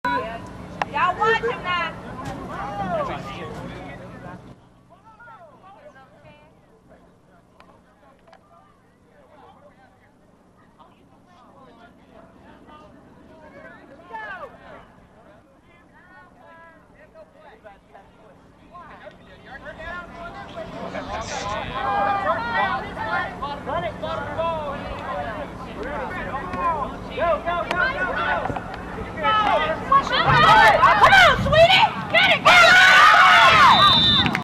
Flag Football: Odd Punt
An odd punt play from a high school girls' flag football game on April 5, 2007.